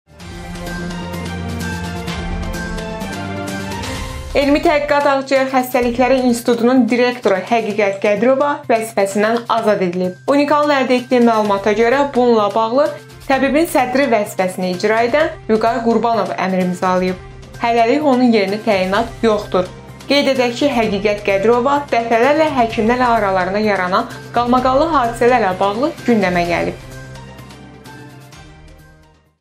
Vüqar Qurbanov qalmaqallı direktoru işdən çıxardı
Elmi Tədqiqat Ağciyər Xəstəlikləri İnstitutunun direktoru Həqiqət Qədirova vəzifəsindən azad edilib.

"Unikal"ın əldə etdiyi məlumata görə, bununla bağlı Tibbi Ərazi Bölmələrini İdarəetmə Birliyinin sədri vəzifəsini icra edən Vüqar Qurbanov əmr imzalayıb.

Hələlik onun yerinə təyinat yoxdur.

Qeyd edək ki, Həqiqət Qədirova dəfələrlə həkimlərlə aralarında yaranan qalmaqallı hadisələrlə bağlı gündəmə gəlib.